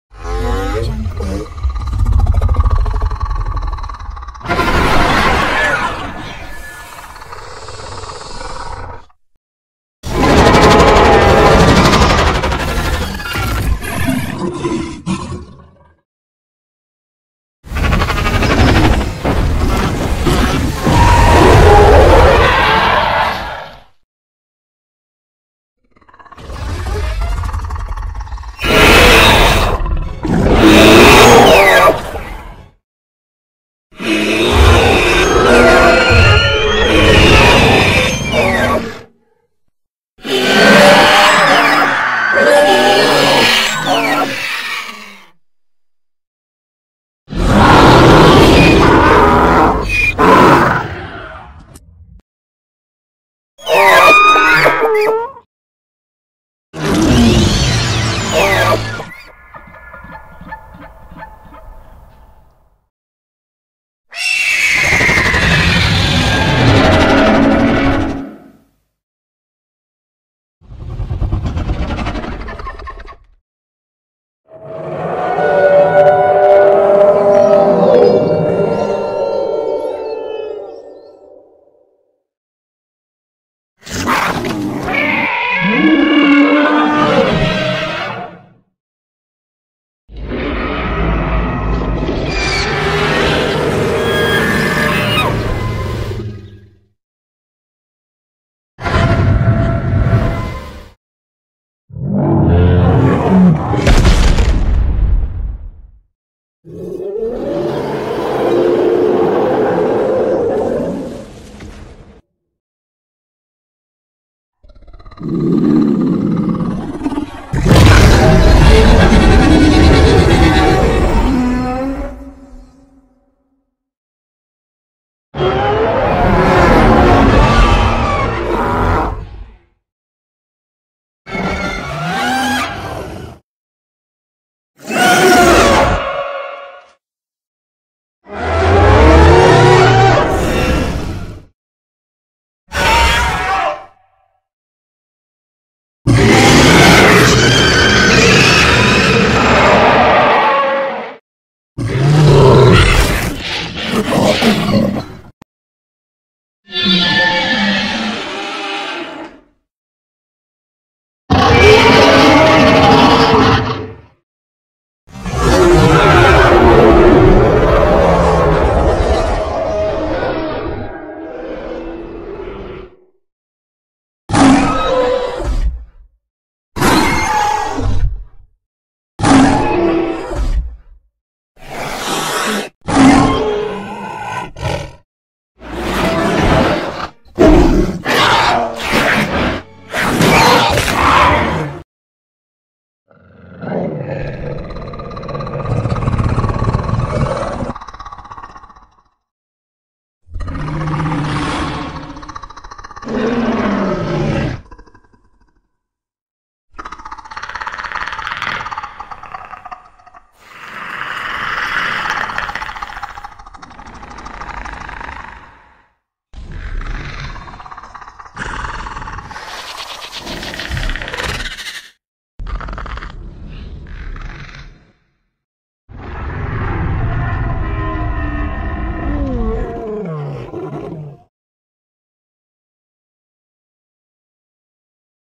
Turning Red's White Lizard Sound Effects
Here is the sound effects of the antagonist monster in Disney and Pixar's Turning Red

The White Lizard

He is a Radioactive ablino reptile that is has dorsals that resamples to Godzilla 1998 ,Head like a Indominus and Long arms and claws like a Therizinosaurus.He is the villan in Turning Red[OC] and the opponent that The G.man will face off to protect Toronto.